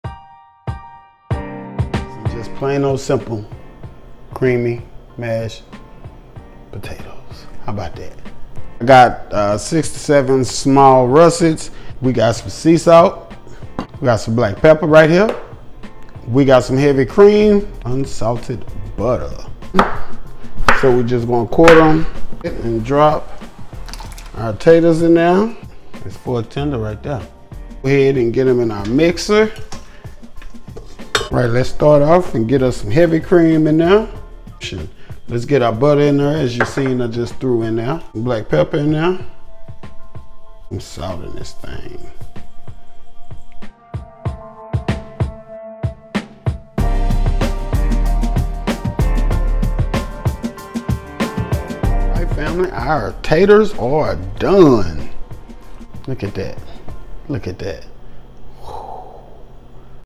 Creamy mashed Potatoes
6-7 small potatoes (quartered)
1 tsp salt (more to taste if needed)
1/2 tsp black pepper
1/3-1/2 cup of heavy cream
2 tbs butter

Over medium high heat in a pot pour 2 1/2 cups of water and bring to a boil. Quarter potatoes and add to boiling water , boil for 15-20 minutes until potatoes are fork tender. Drain water and add to a mixing bowl using a hand mixer or stand mixer blend until smooth and creamy add salt and pepper to taste. Plate and enjoy!!!